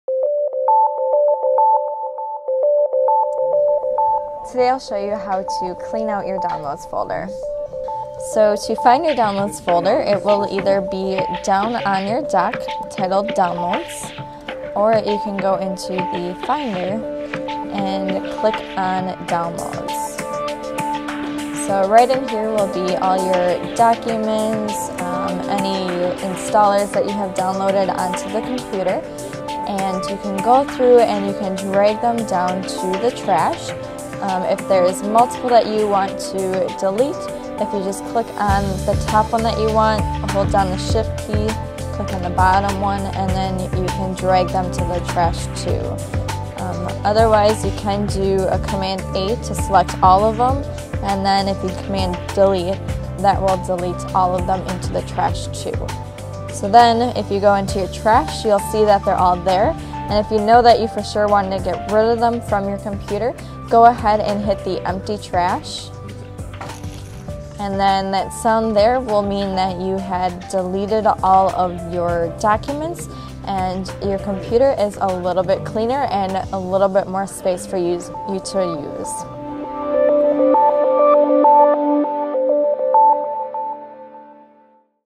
How to Clean Out Your Downloads Folder and Trash
Christina shows how to clean out your downloads folder and empty your trash to help create space on your computer and help speed up programs running.